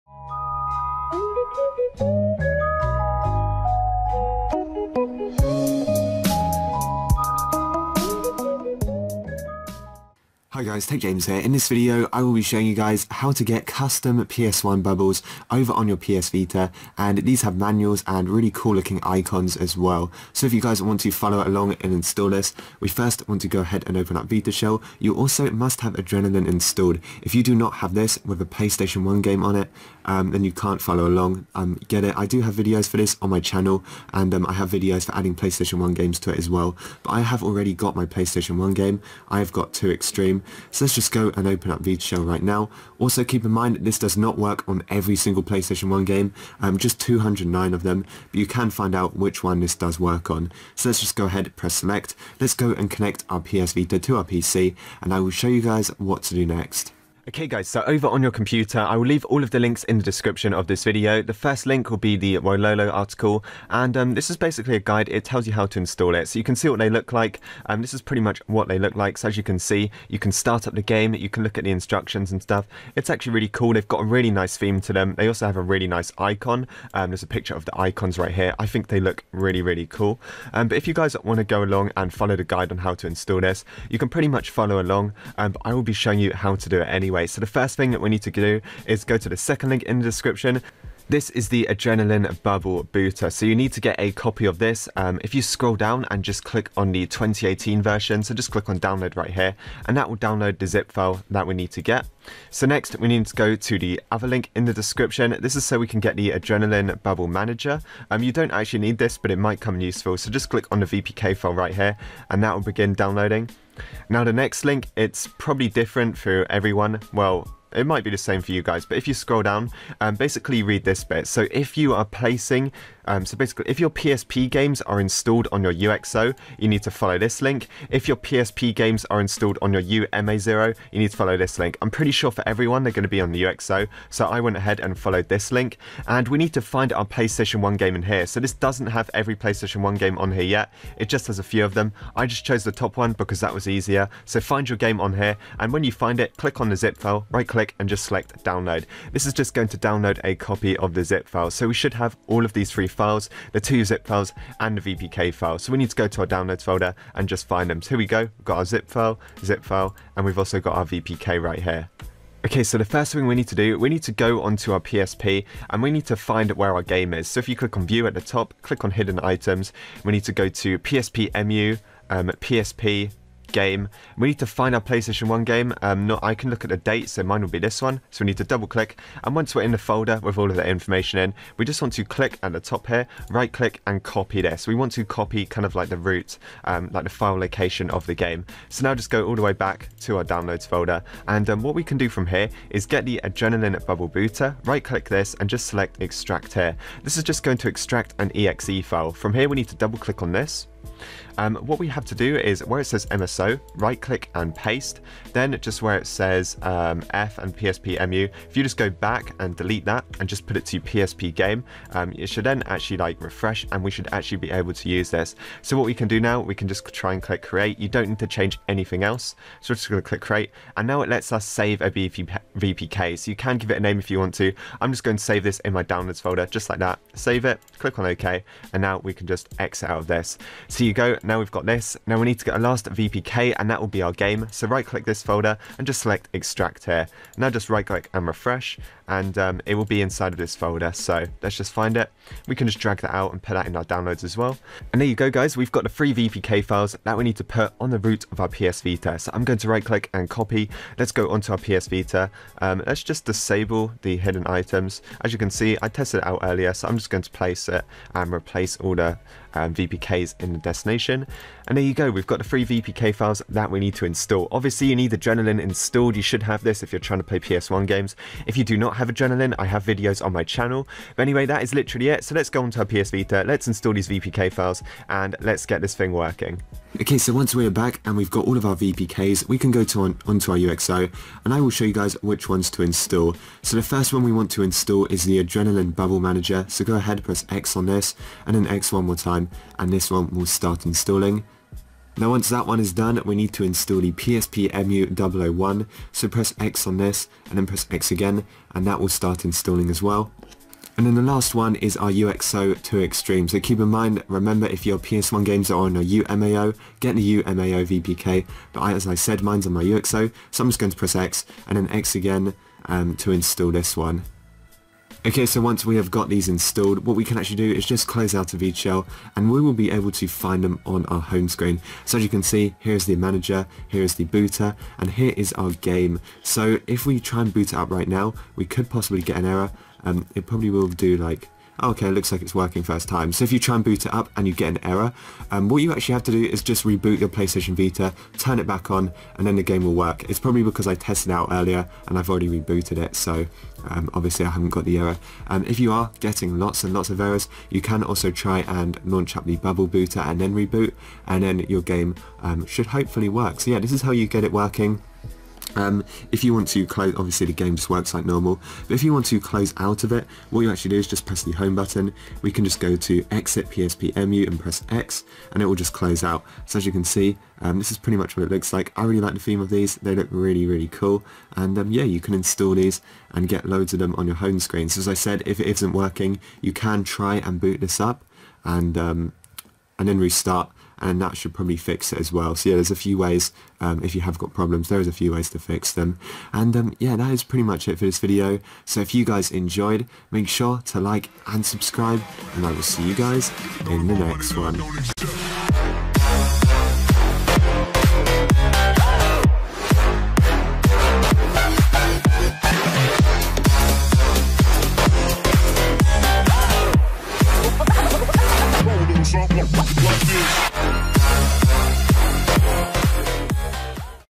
PS Vita Install Custom PS1 LiveArea bubbles!
How to install improved PS1 Bubbles on your PS Vita! You must have Adrenaline installed to do this! You also need a PS1 game of your choice, must be installed on your UX0 or UMA0. This was made by Belickade!